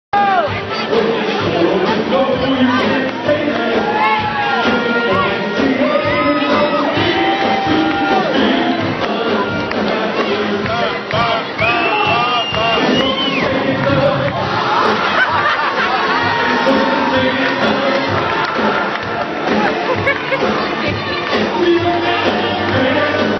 Bride rides mechanical bull in wedding gown
A bride in full wedding attire mounts the mechanical bull. What happens next is just plain hilarious. Check it out!